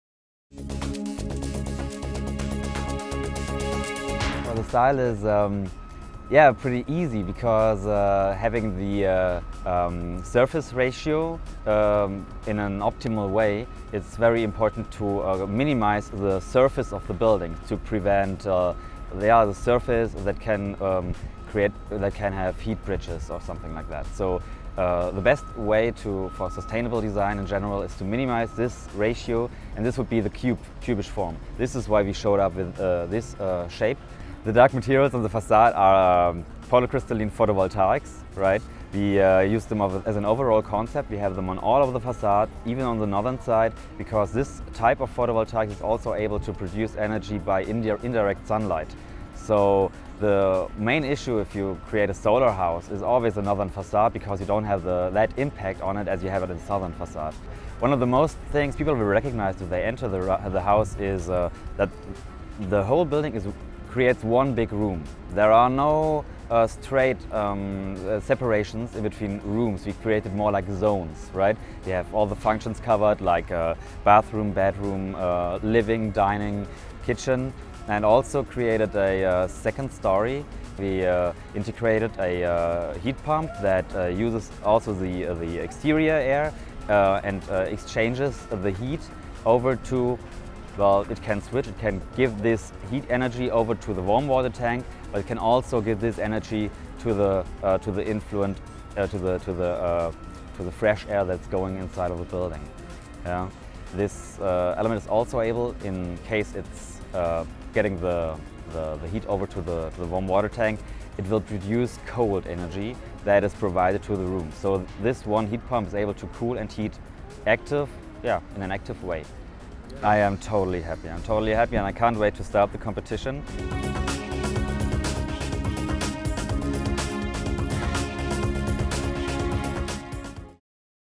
Team Germany Virtual Tour
Team Germany's solar house won first place overall in the 2009 Solar Decathlon.